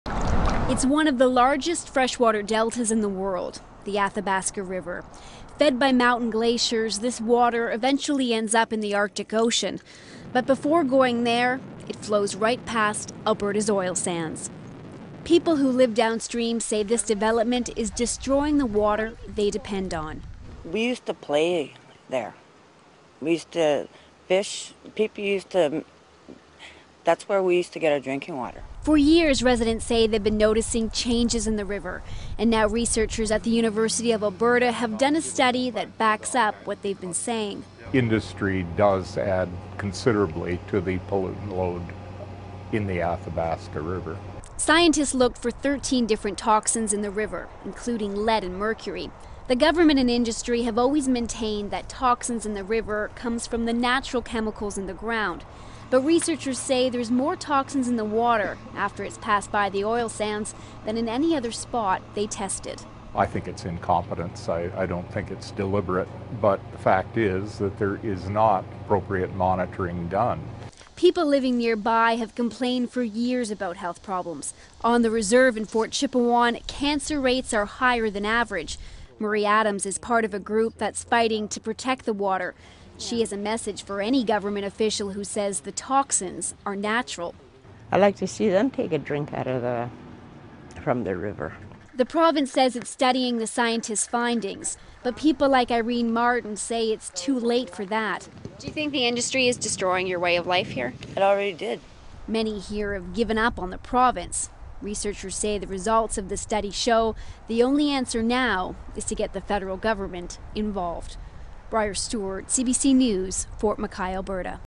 Alberta Canada's Oil Sands: Pollution and Cancer Rates
The Canadian Government and industry have always maintained that the toxins from the river come from the natural ground chemicals.

Cancer rate are higher for the locals.

Lead and mercury where mentioned and are toxic for the brain and nervous system but the report did not mention Benzene, a known cancer causing chemical.